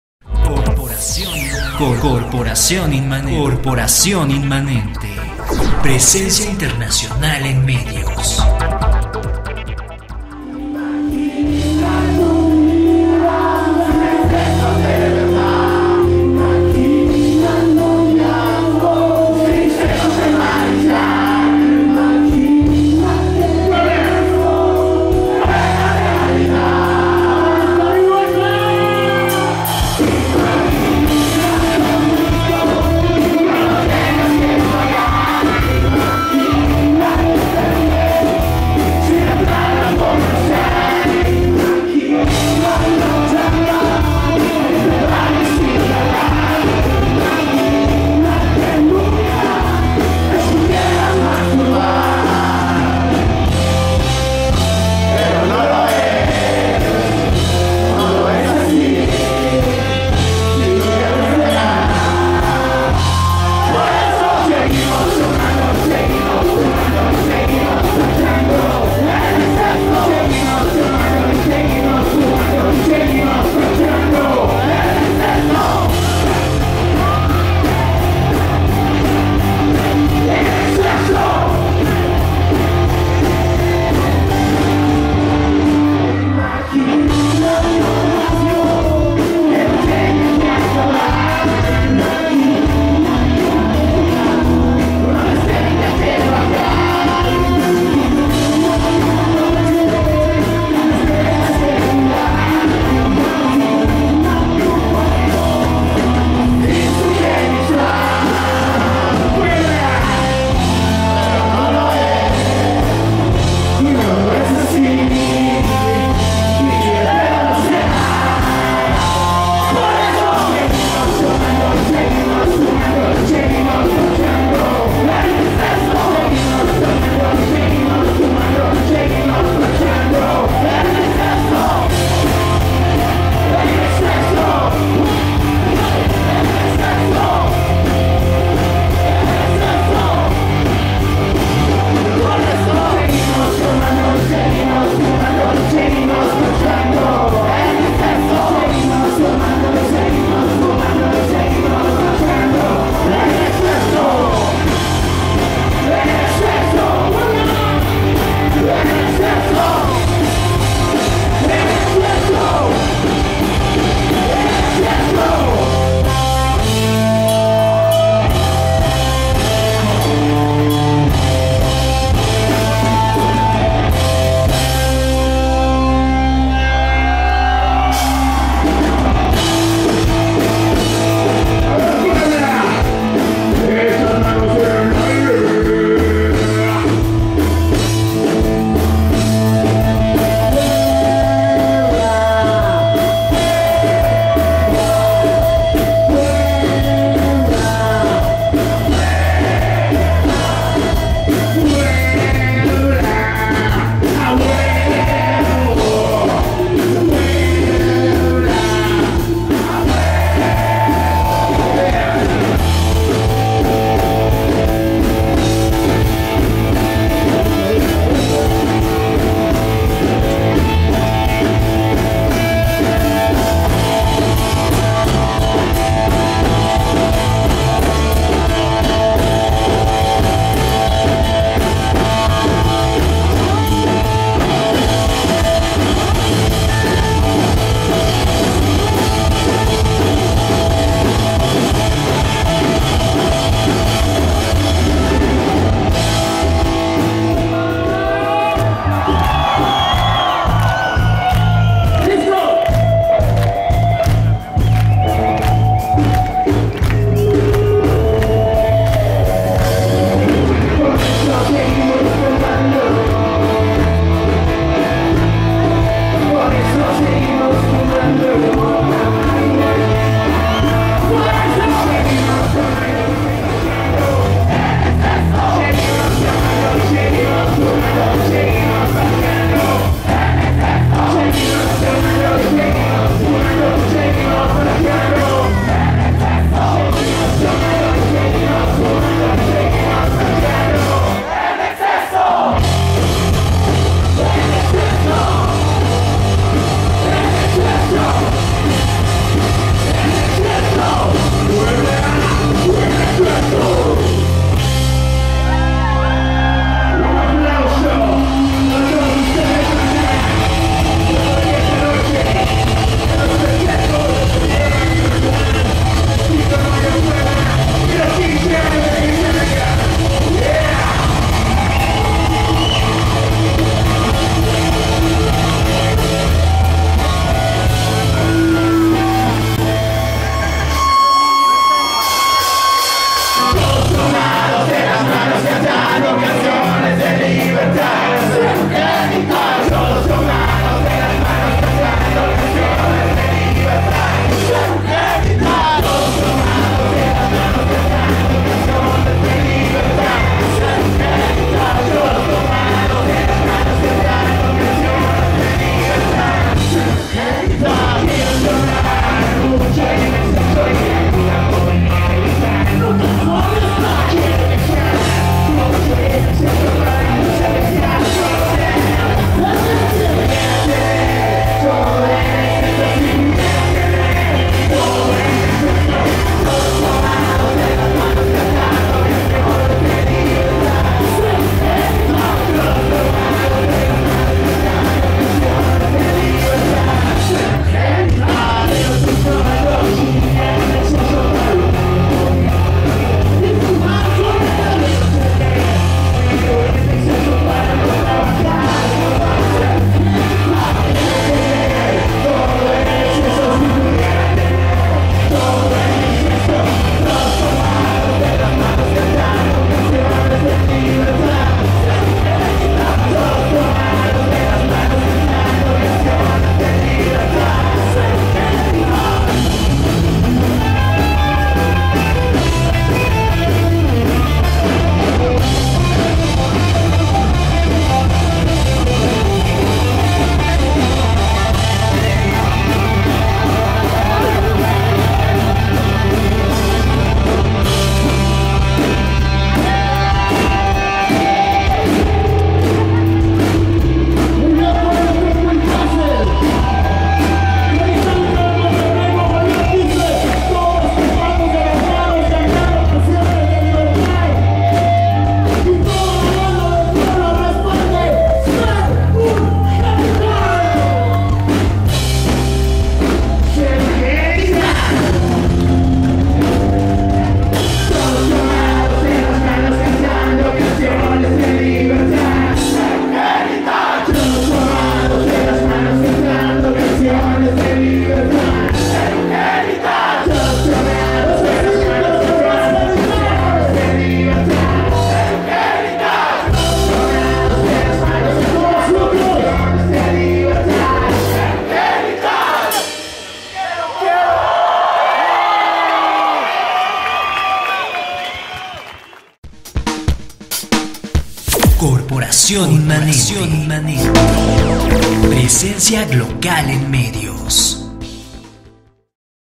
Presentacion de Genitallica en Puebla parte 2
Les compartimos parte de la presentación del grupo Genitallica en pasado 04 de mayo en el foro juvenil